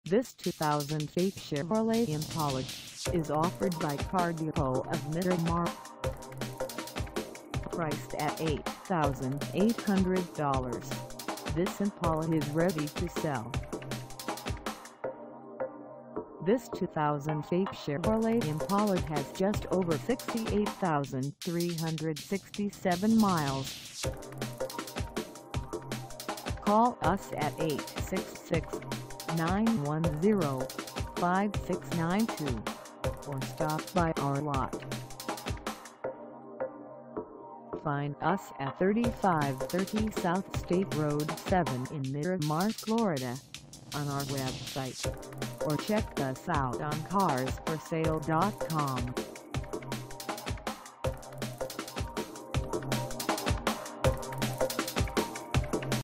2008 Chevrolet Impala LS Sedan for sale in Miramar, FL 33023
This 2008 Chevrolet Impala LS Sedan is for sale in Miramar, FL 33023 at Car Depot of Miramar.
Engine: V6 3.5L V6
Transmission: Automatic 4-Speed
Color: Unspecified
Mileage: 68367
Contact Car Depot of Miramar 3530 S STATE ROAD 7 Miramar, FL 33023 to test drive this 2008 Chevrolet Impala LS Sedan today.